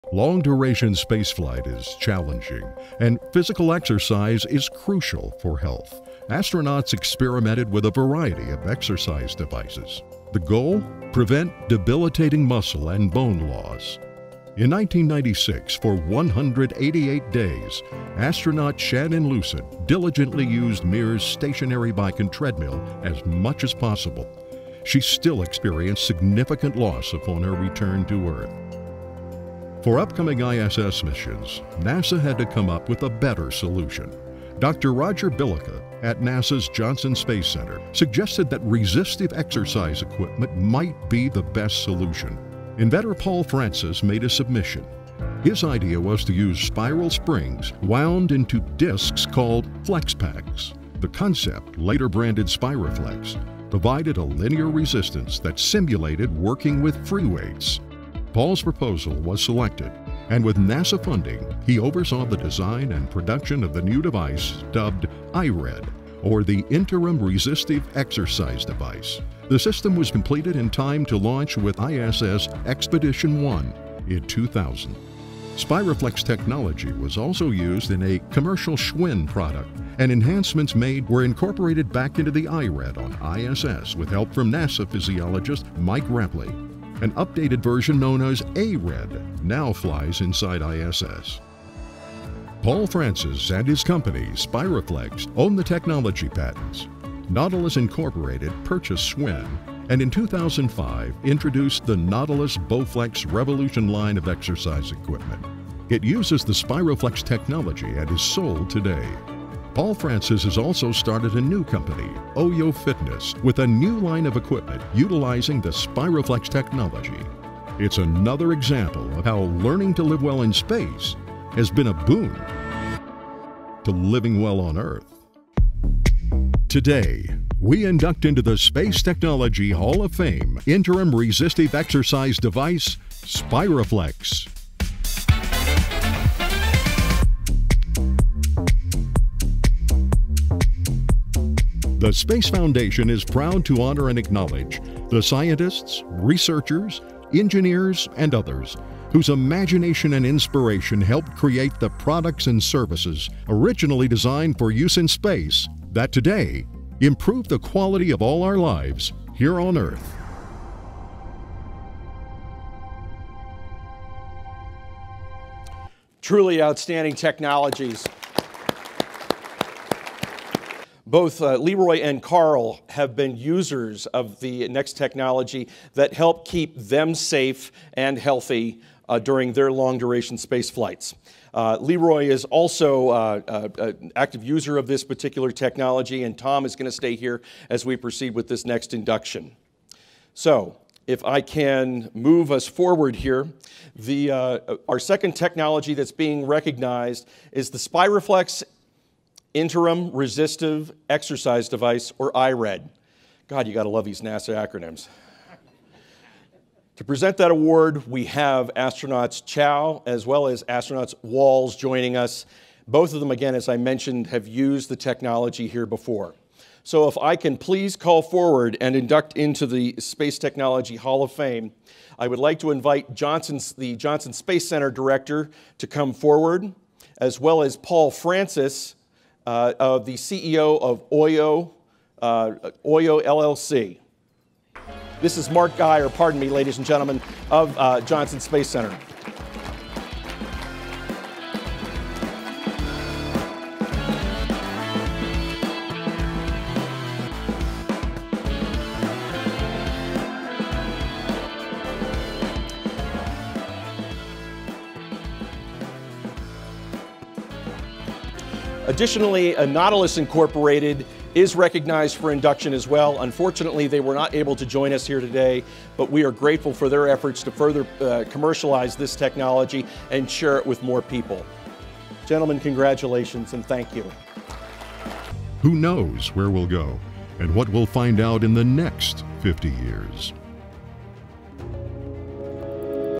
SpiraFlex technology inducted into the Space Technology Hall of Fame
Astronauts lose bone and muscle in space in zero G very quickly. It was mission critical to find a countermeasure for astro health and ability to work when arriving on the Moon or Mars. NASA's research confirmed free-weights are the best way to build muscle and bone on earth. The problem was finding the same resistance properties that would work in space. I presented my SpiraFlex resistance technology to NASA. NASA conducted a 16 wk study on earth and found SpiraFlex provided the same benefits as free-weights. It has been used for 10 year on the International Space Station to keep astronauts fit. This same SpiraFlex technology enables the OYO and NOVA Gyms to replace free-weights, yet only weighs 2 lbs.